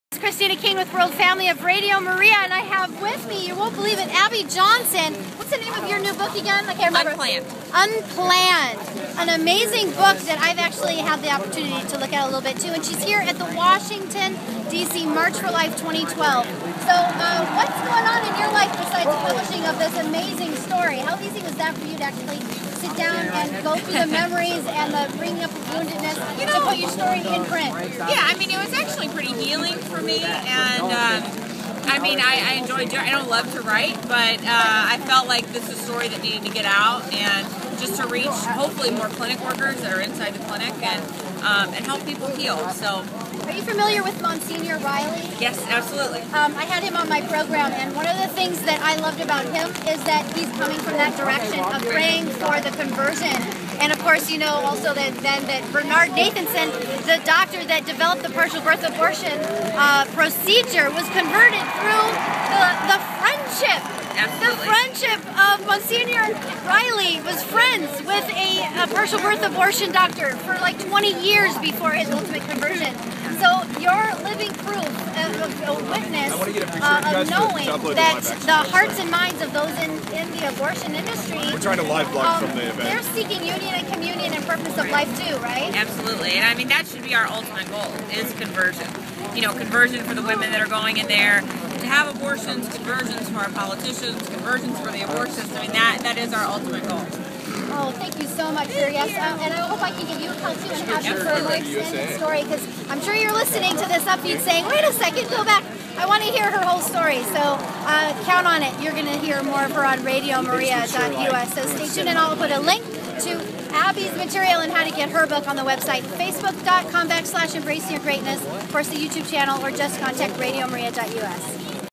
MARCH FOR LIFE 2012; Abby Johnson
A woman who used to be a part of the abortion industry speaks out!